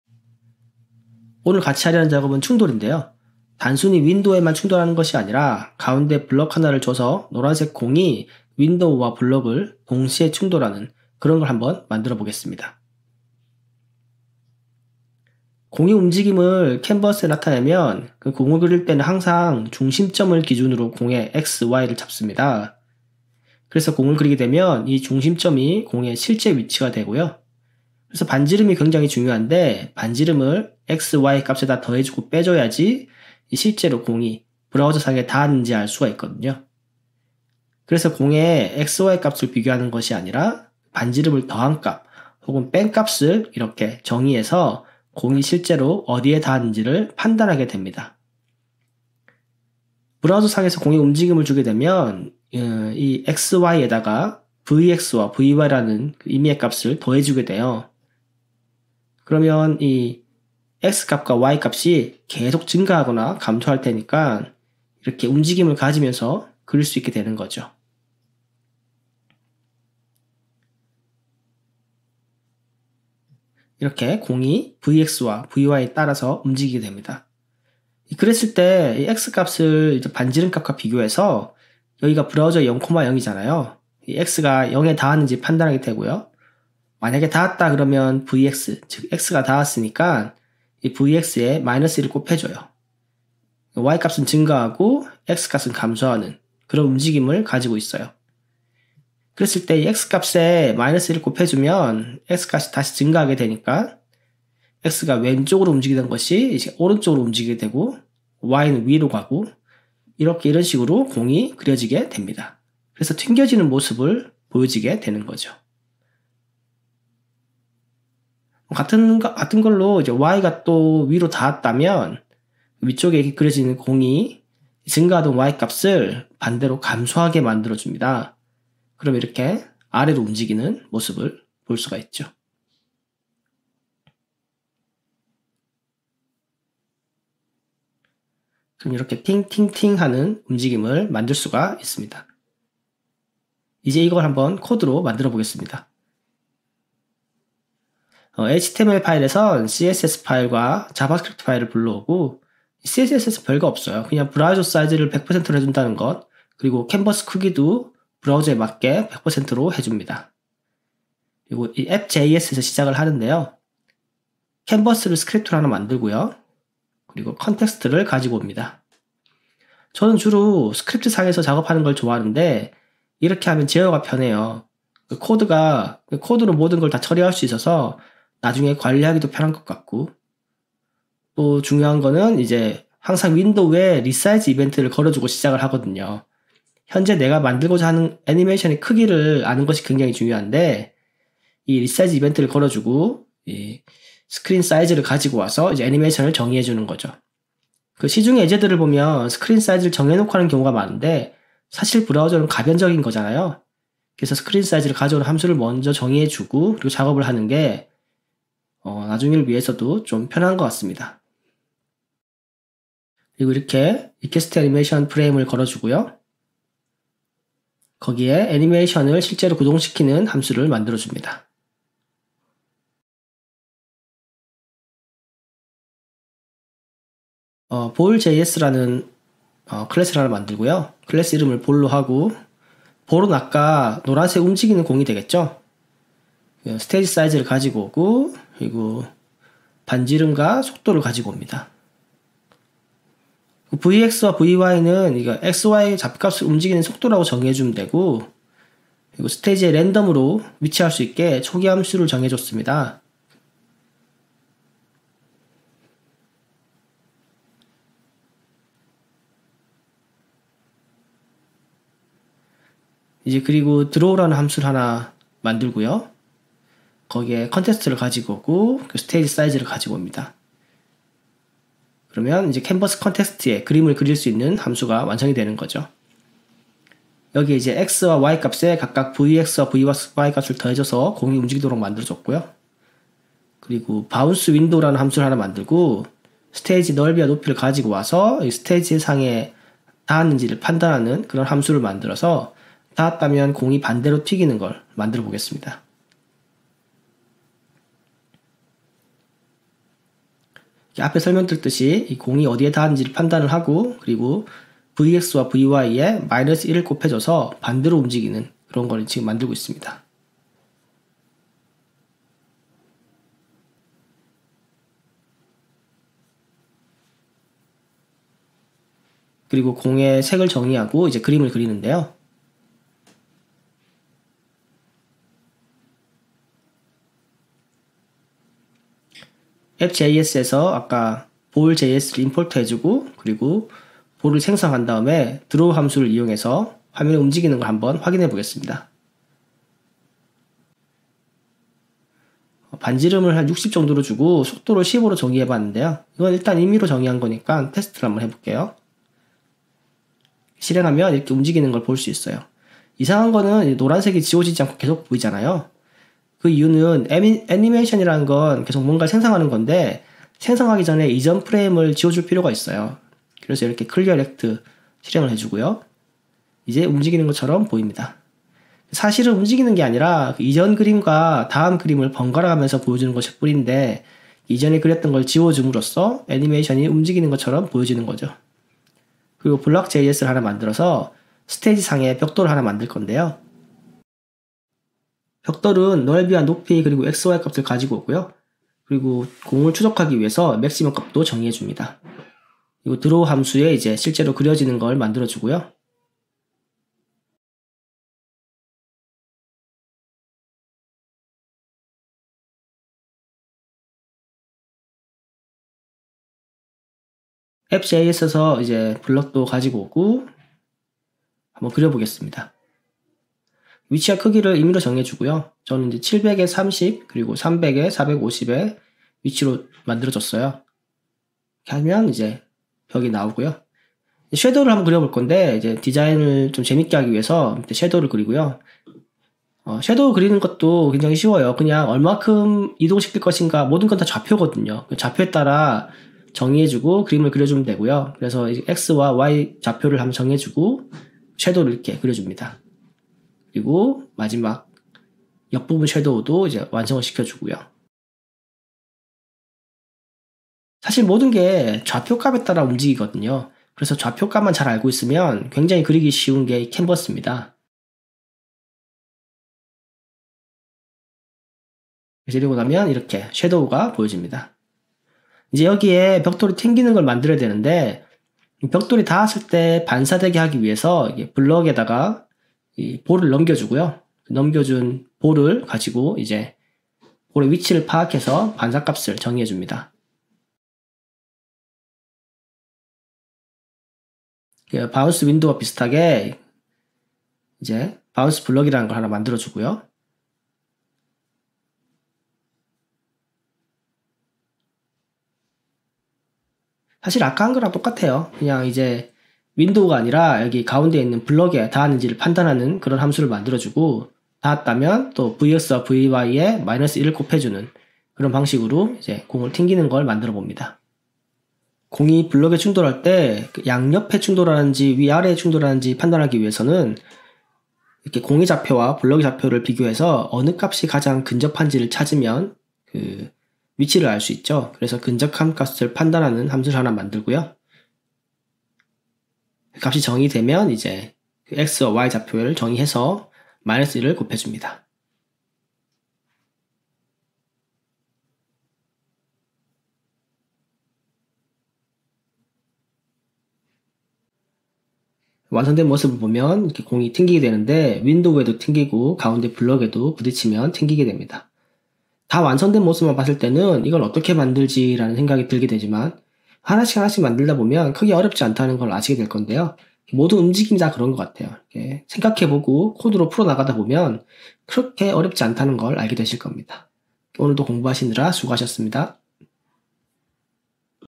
Creative Coding Tutorial : Ball Collisions and Bouncing with JavaScript
It creates a moving ball in the screen and when it collides with the object, it causes it to bounce in the opposite direction. This method is used a lot in games, interactions, etc., and you can see the principles of animation made with code.